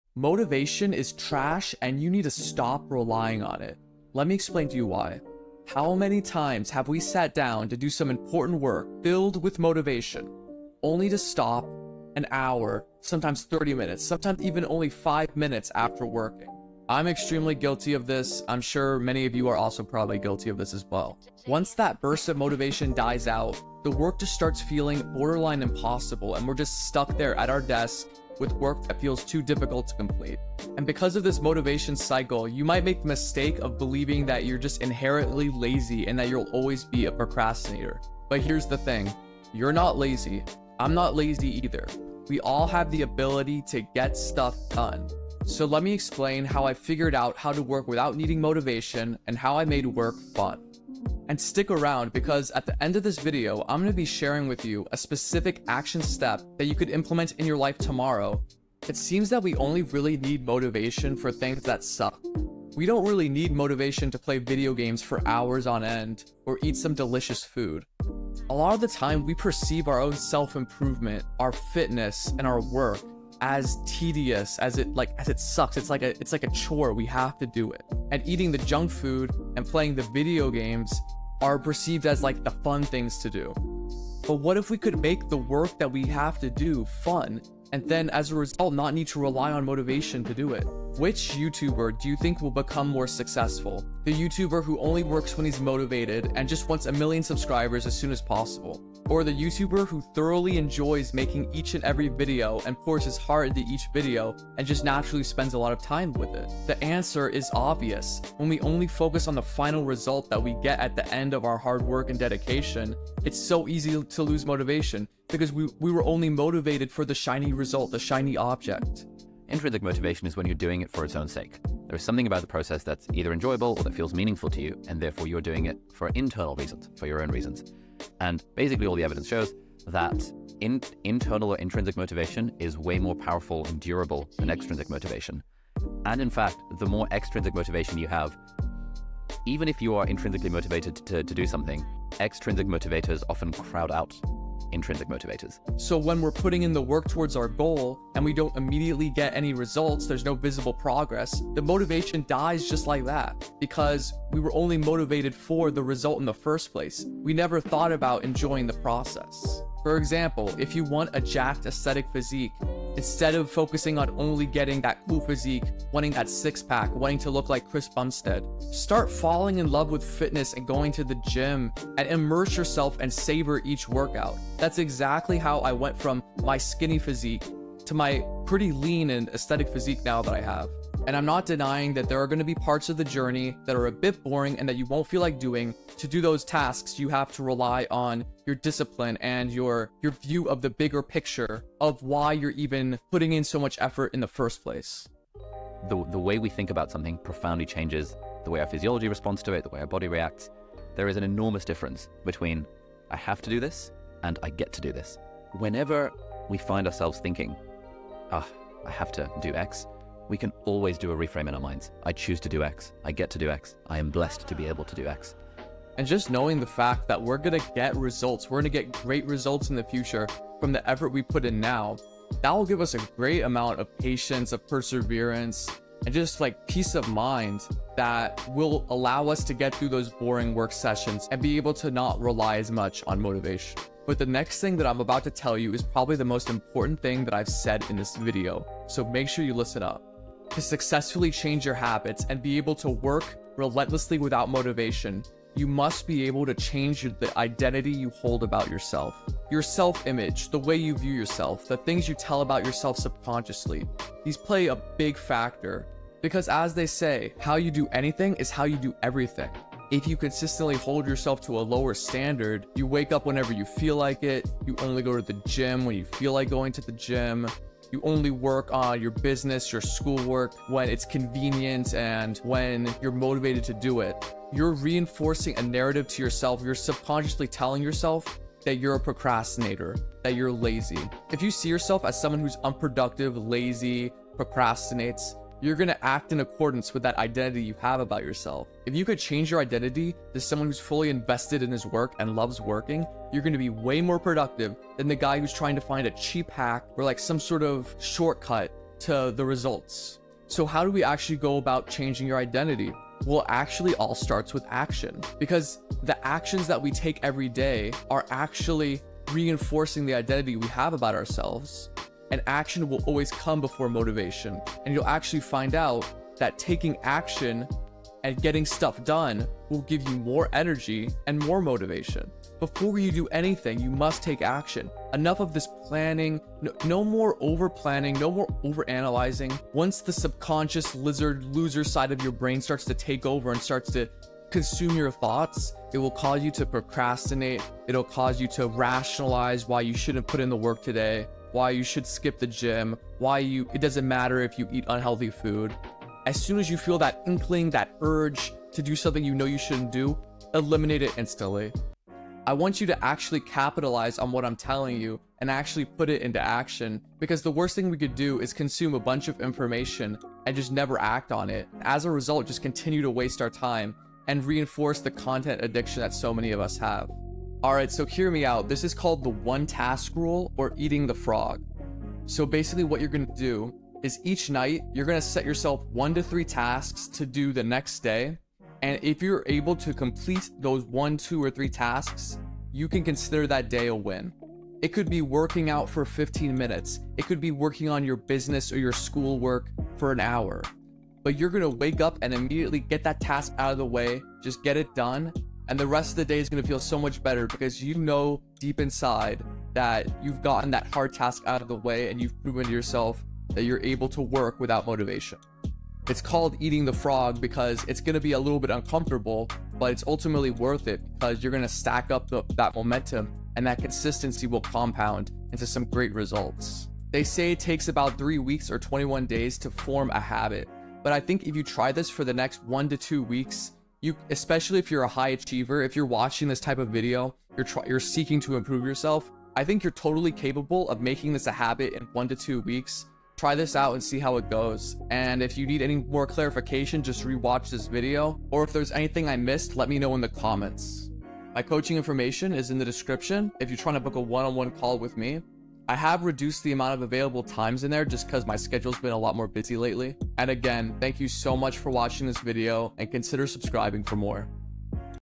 How to work without motivation.
Motivation doesn't get you far. Here's how to get stuff done without it.

Chapters:
0:00 - Intro
0:58 - How to make work fun/enjoy the process
3:53 - Change your identity/self-image
5:51 - The One Task Rule (try out this exercise)